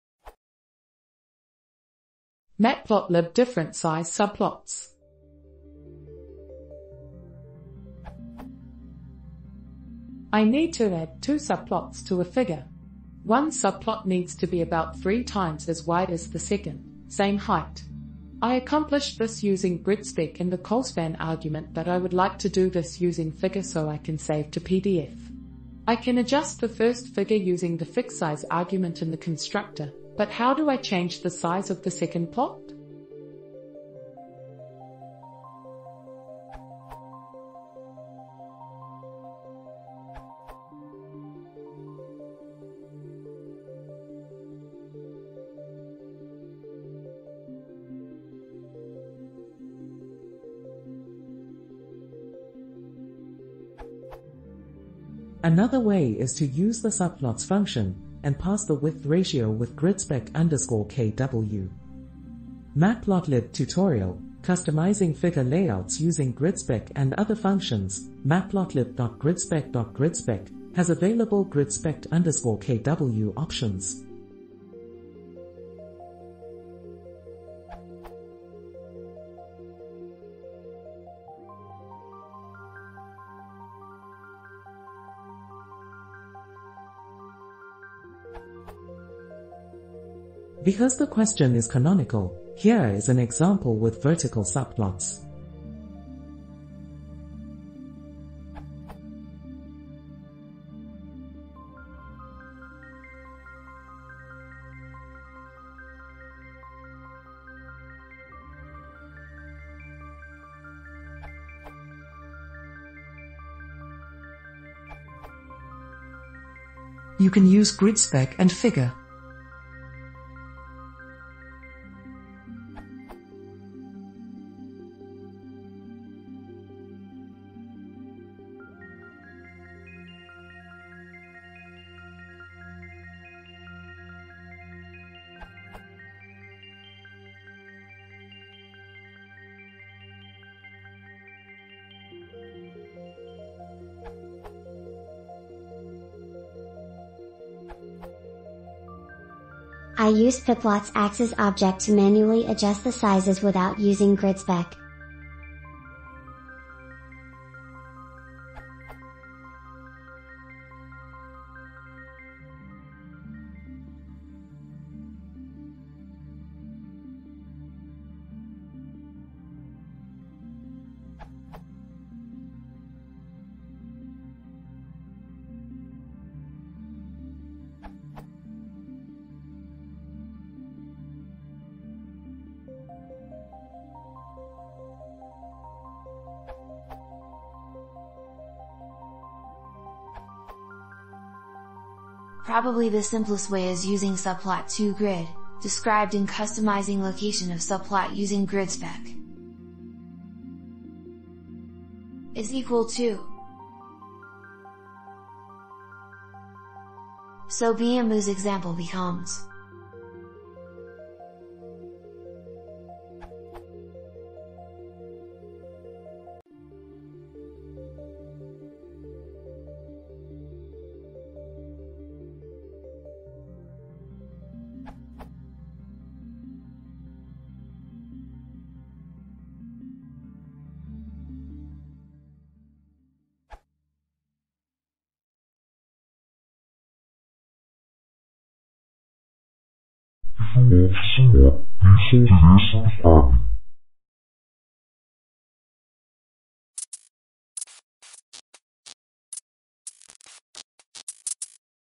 Matplotlib different size subplots
Chapters
00:00 Question
00:37 Accepted answer (Score 596)
01:51 Answer 2 (Score 266)
02:24 Answer 3 (Score 40)
03:07 Answer 4 (Score 38)
03:54 Thank you